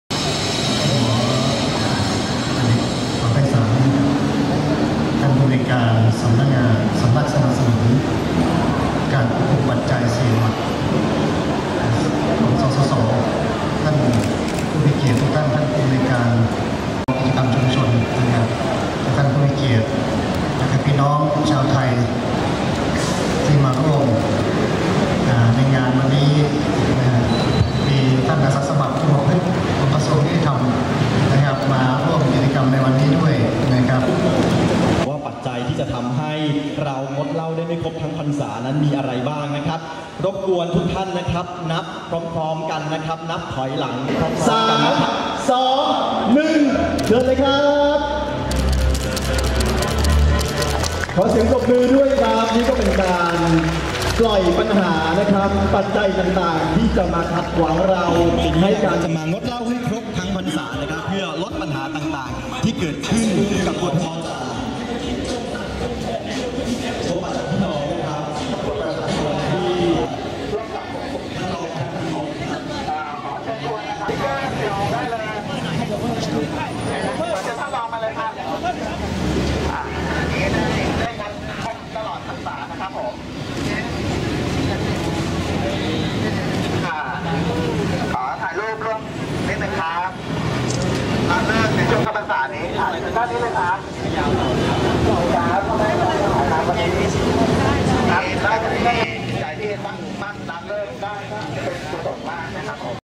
สัญญา งดเหล้าครบพรรษา
นายเกษม มูลจันทร์ รองอธิบดีกรมคุมประพฤติกระทรวงยุติธรรมเป็นประธานในการรณรงค์"สัญญา...งดเหล้าครบพรรษา" พร้อมรณรงค์ร่วมกับสำนักงานกองทุนสนับสนุนการสร้างเสริมสุขภาพ(สสส.)มูลนิธิหญิงชายก้าวไกลและภาคีเครือข่าย100คนแจกสื่อให้ประชาชนทั่วไปพนักงานขับรถโดยสารและร้านค้าบริเวณป้ายรถเมล์รอบอนุสาวรีย์ชัยสมรภูมิ ภาพ ปราโมทย์ พุทไธสง(Pramote Putthaisong)nationphoto